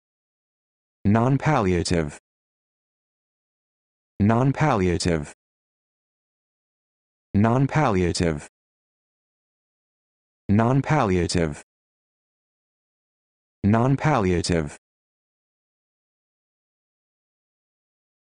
How to Pronounce Nonpalliative
This video shows you how to pronounce Nonpalliative